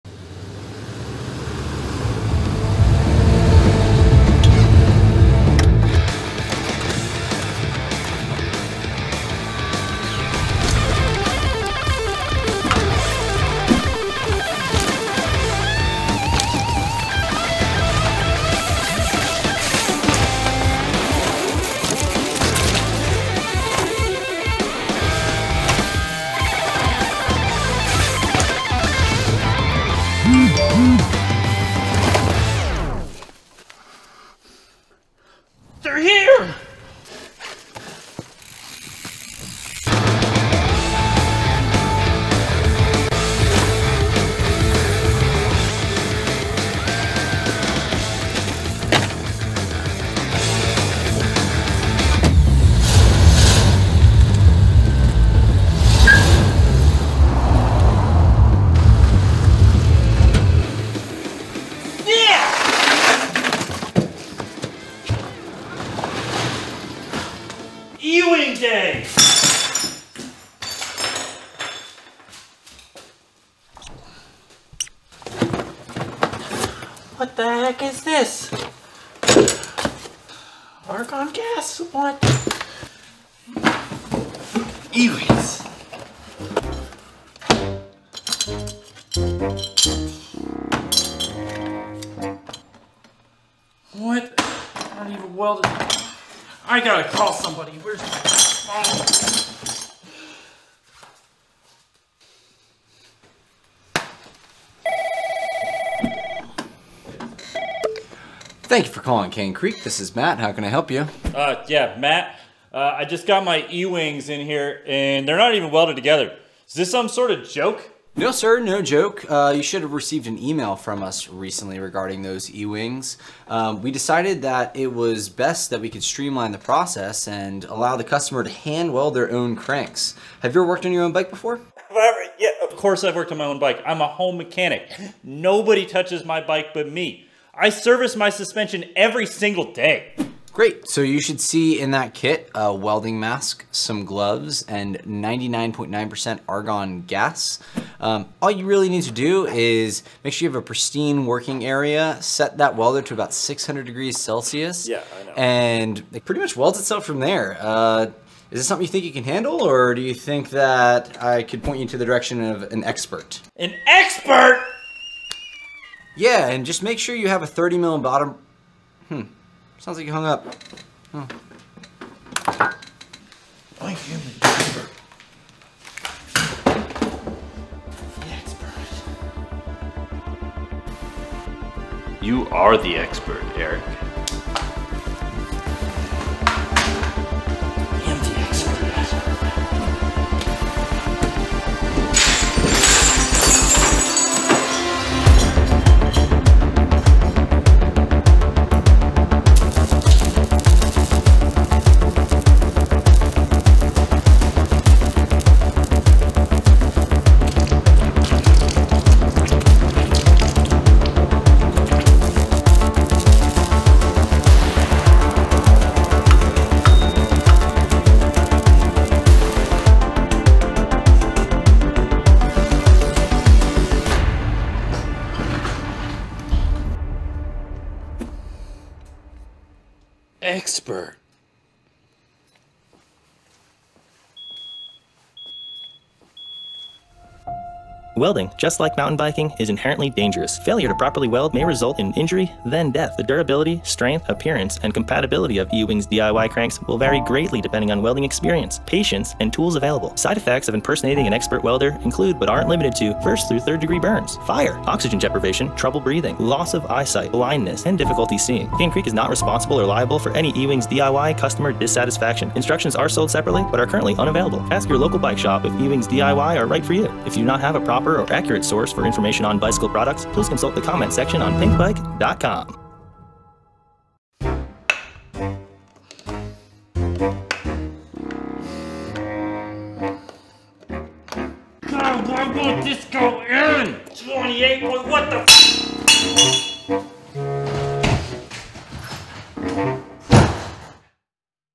eeWings DIY
Cane Creek is proud to announce the latest addition to our lineup of acclaimed eeWings Titanium cranks- the eeWings DIY. The eeWings DIY arrives in 9 individual unwelded pieces consisting of the same 3AL-2.5V and 6AL-4V titanium as the original eeWings crankset. The kit can then be assembled and welded by the rider - at home or wherever you have advanced welding equipment.

The eeWings DIY are a perfect addition to Cane Creek’s premium crank line up. They are aimed at riders who prefer to service and customize their own bikes to a level typically only possible in advanced manufacturing settings.